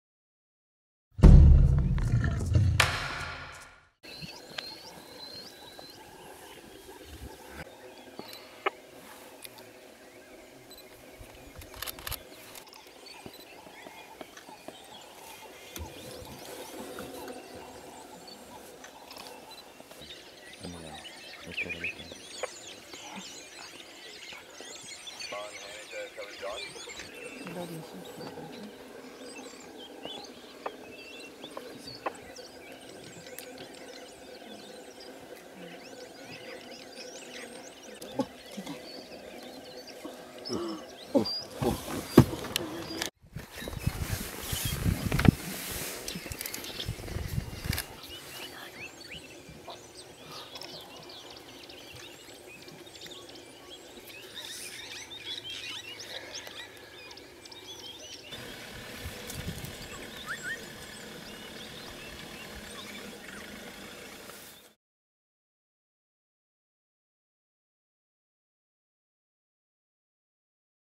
Cheetah Takes Down Nyala - Londolozi TV
A male cheetah stalks and kills an Nyala in the Sand River on Londolozi Game Reserve.